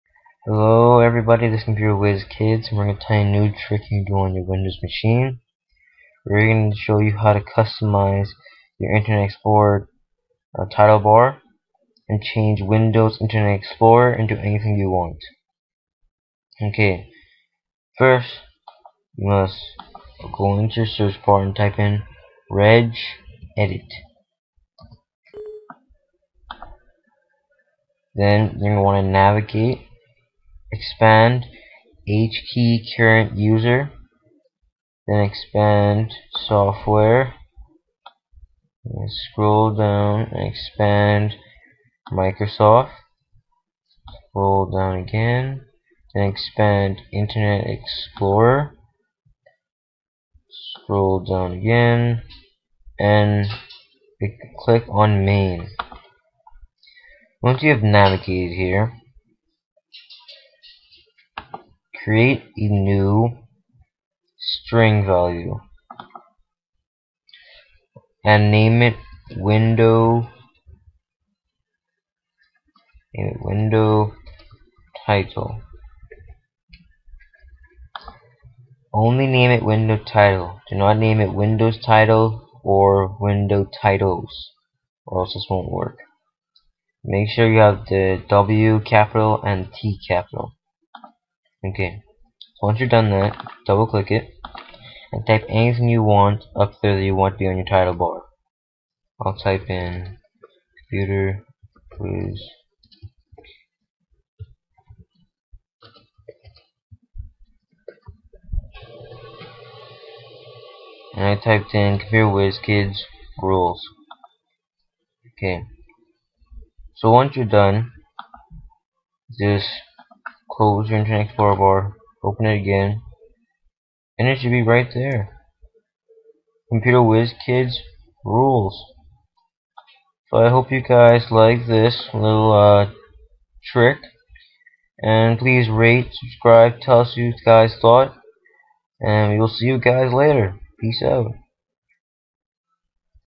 How to Customize the Internet Explorer 8 Title Bar
Hello youtubers this is a video showing you how to change your Internet Explorer 8 title bar to what ever you like.